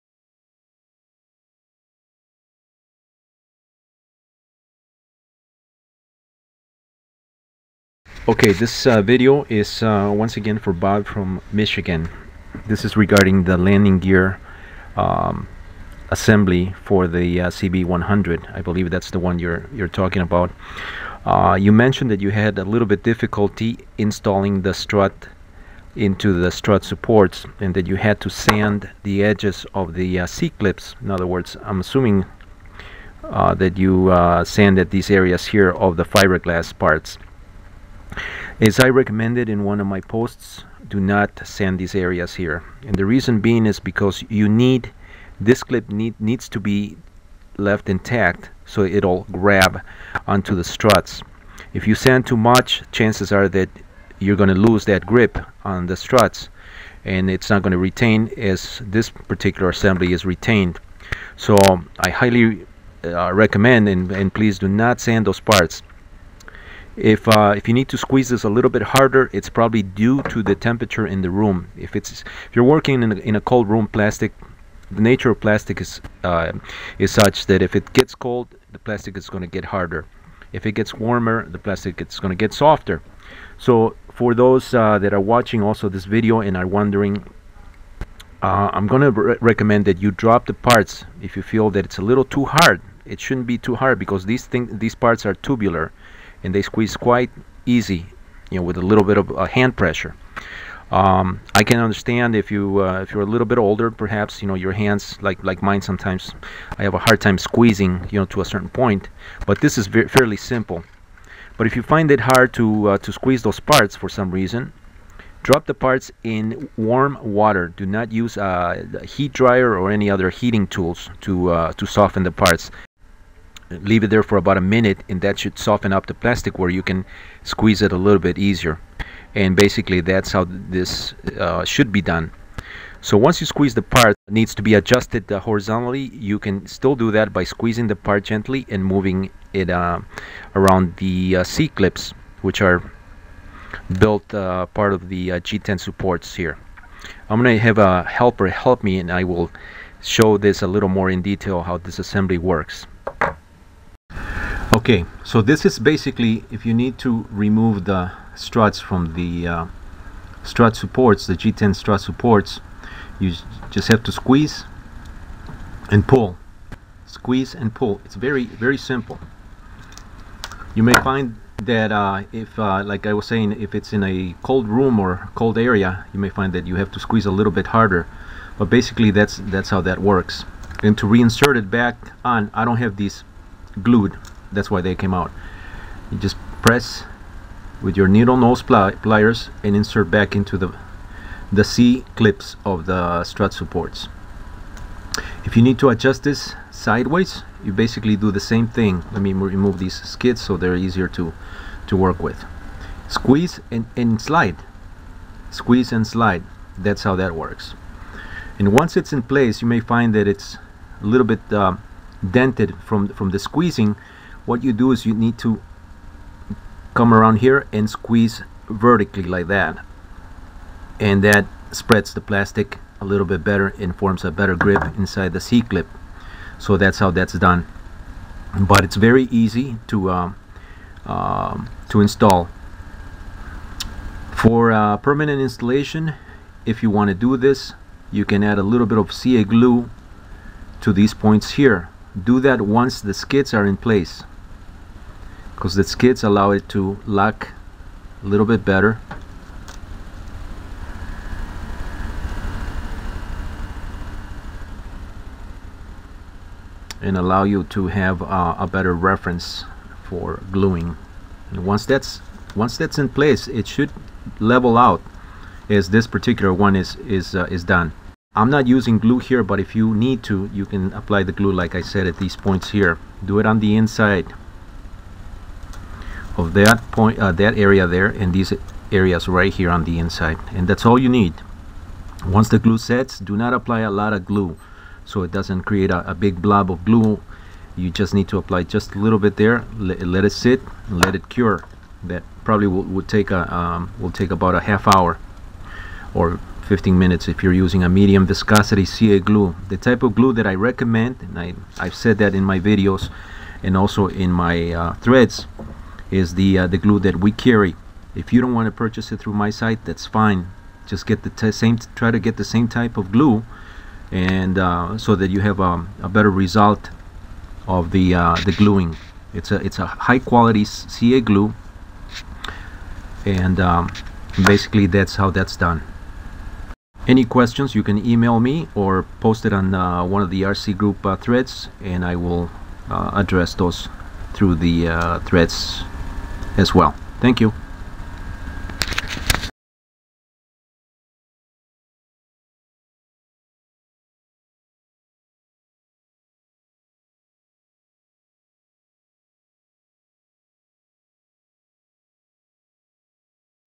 Car Key Chain Video Camera - MIA TUFF (TM) Landing Gear Strut Installation - Walkera CB100
Proper way to install the MIA TUFF (TM) Landing Gear Struts. Taking advantage of the car key chain video camera. This video was made for a customer of mine to clear up some small details. This video is also for anyone that has purchased or wished to purchase a MIA TUFF LG for the Walkera CB100, 4#3, 4G3, 4G6, Hirobo Quark, Nine Eaqles, and E FLite Blade mSR helicopters. The installation is same throughout all MIA TUFF (TM) LG Upgrades for all helicopters, since the approach is the same we have been employing for many years since the first MIA upgrades we made for the earliers helicopters such as the Piccolo, Hornet , etc.
The MIA TUFF(TM) LG Upgrade was developed from an original aplication done  initially to the 1999 MIA Robin 280 helicopter  and similar  MIA Helicopter Kits including the MIA House FLY, MIA Bumble Bee, MIA Mosca.